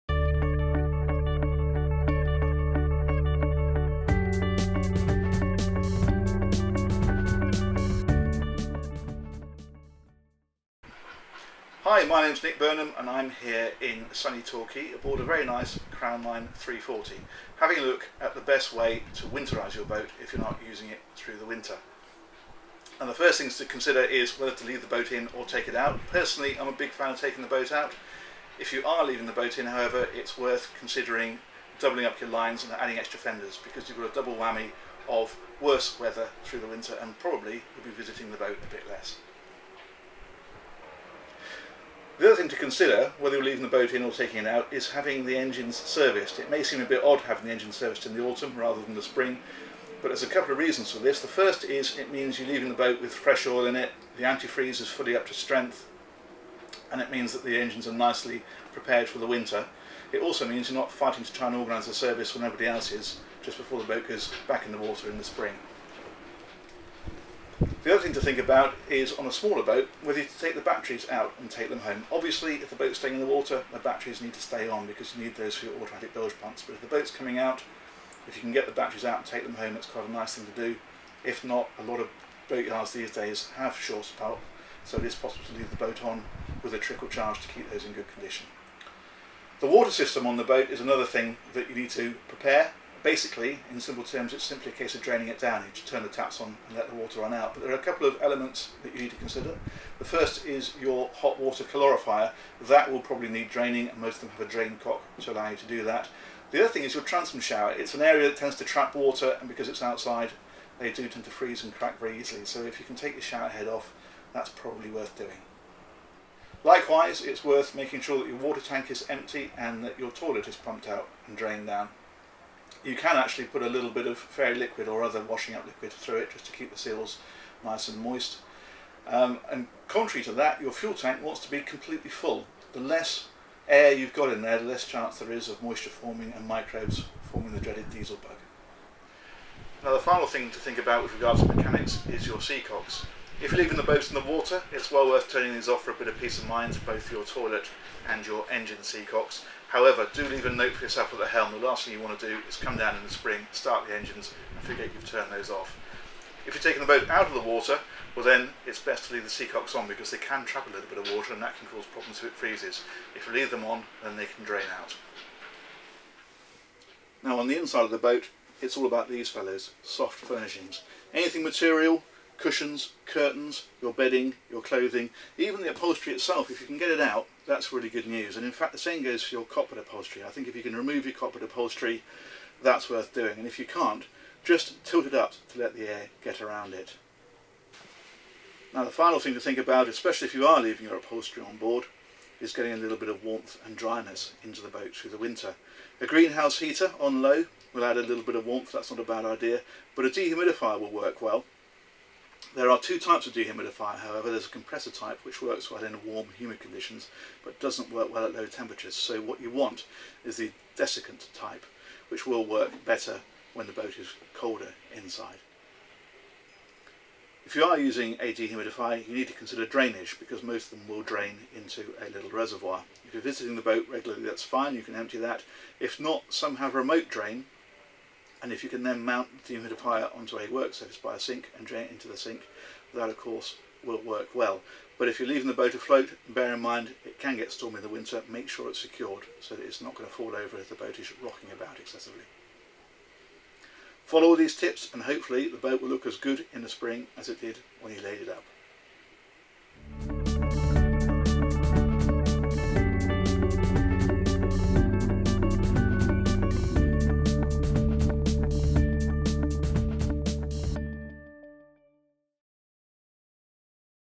How to... Winterise a boat | Motor Boat & Yachting
A step-by-step video guide showing you how to prepare your boat for winter.